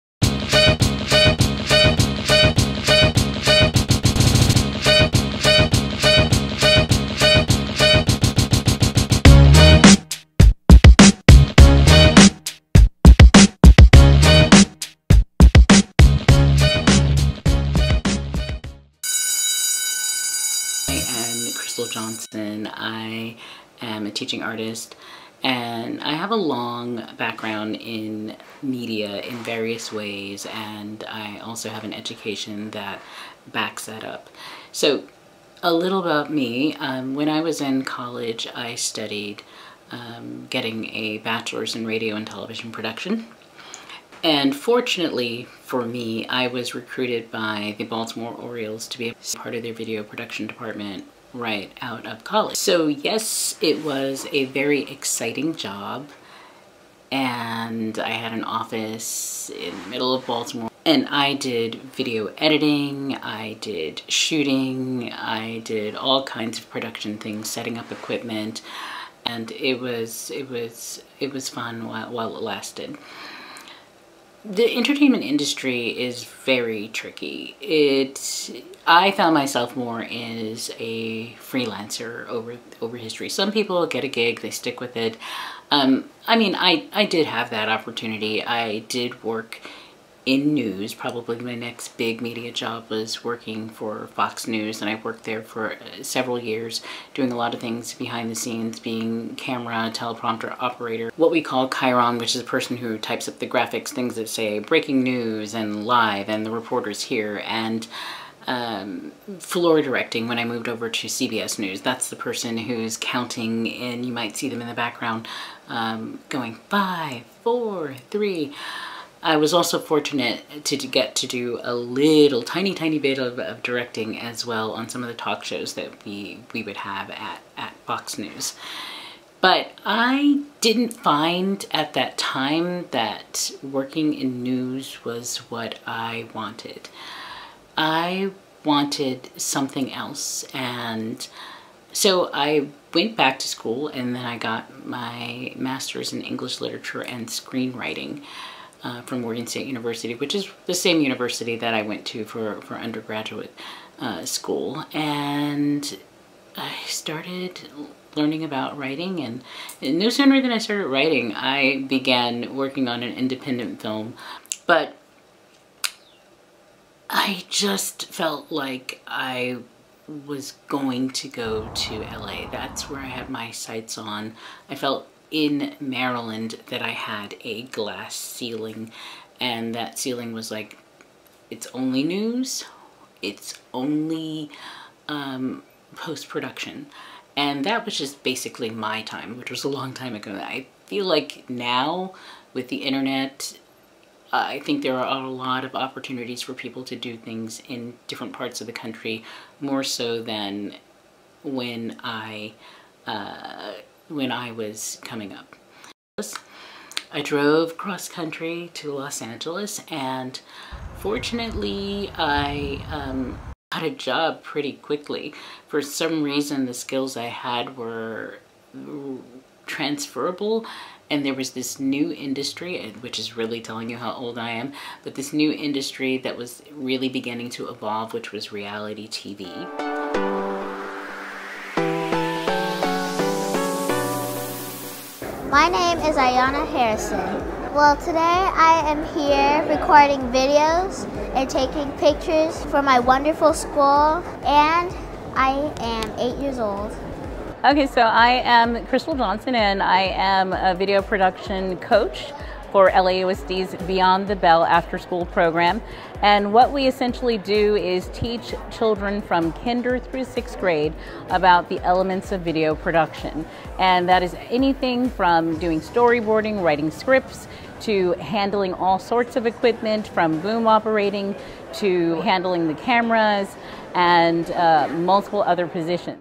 Meet the Instructor
The M.E.T.A staff includes people with media industry experience and in the case of Crystal Johnson, graduate study in English Literature/Screenwriting.   She's part owner of M.E.TA and excited to teach your child.